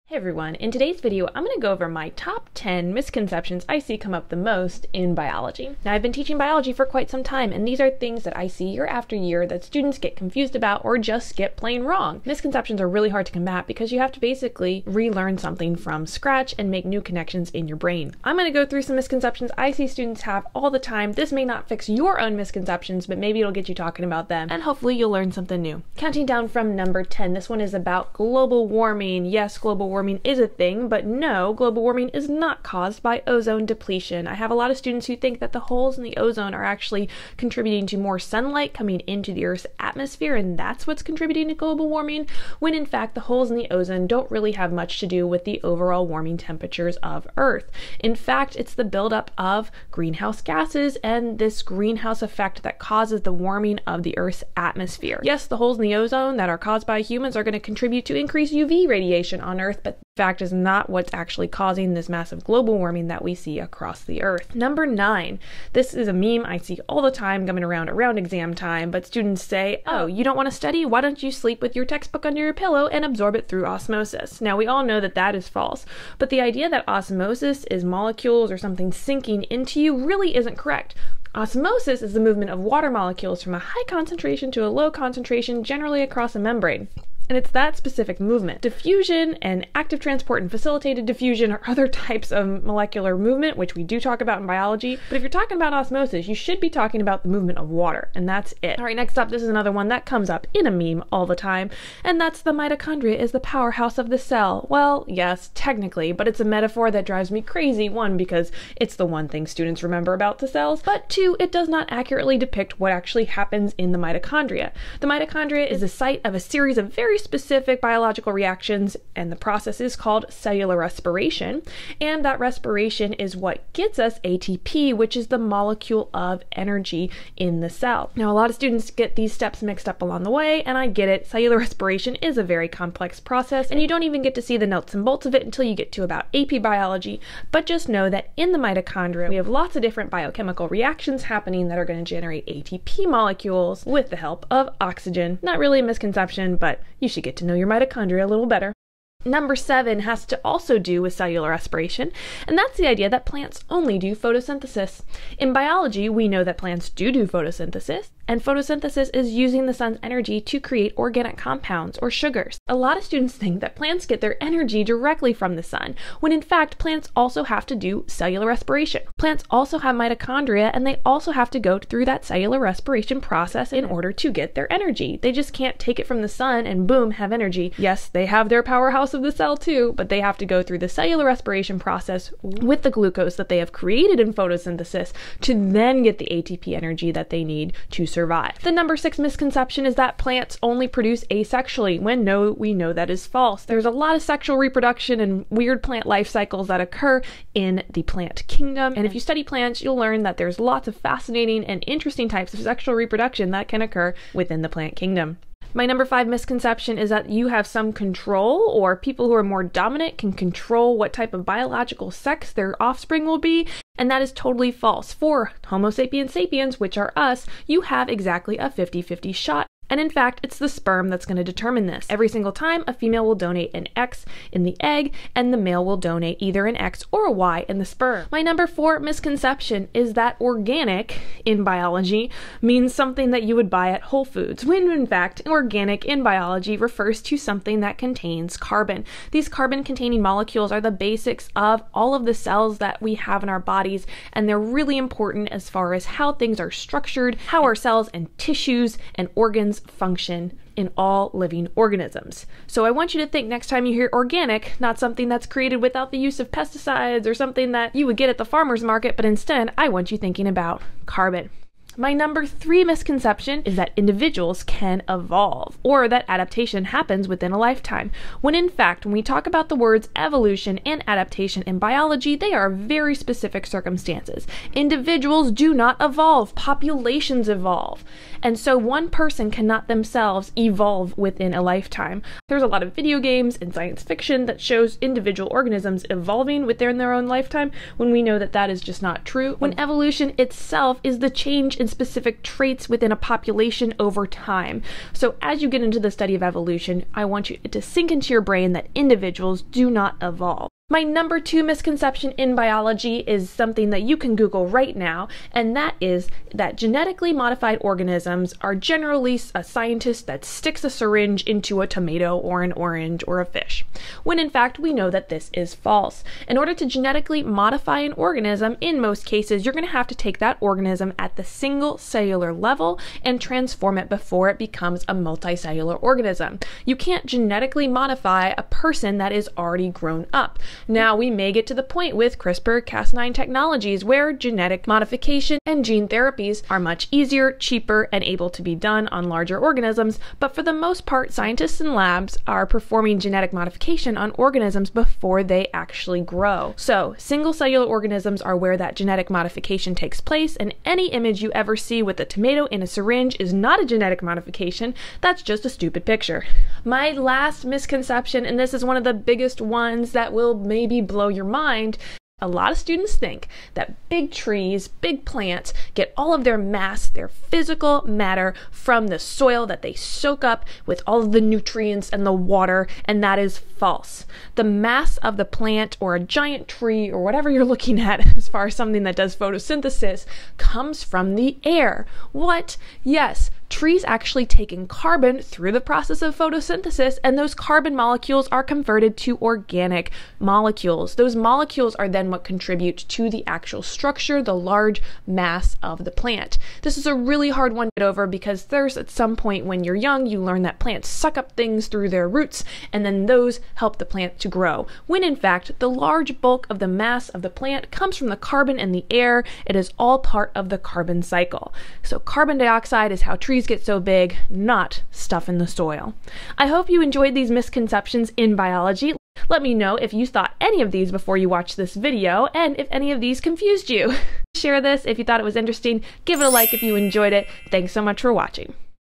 Top 10 Student Misconceptions in Biology Class
What are some of the most common misconceptions students have about Biology? Are there any science facts you learned in school that you later found out weren't true?

In this video, I'll share the top 10 misconceptions I come across the most in my Biology classes. If you're a teacher or student watching, share any misconceptions you've seen in Biology classes.

00:00 Why are misconceptions so hard to un-learn?
00:26 10) Global Warming & the Ozone Layer
01:04 9) Osmosis & Absorption
01:39 8) Mitochondria is the Powerhouse of the Cell
02:29 7) Plants & Cellular Respiration
03:08 6) Is plant reproduction asexual?
03:26 5) Can you control the biological sex of your offspring?
03:48 4) What does organic mean in biology?
04:22 3) Can individuals evolve?
05:00 2) What does genetic modification look like?
05:47 1) Where does the mass of a plant come from?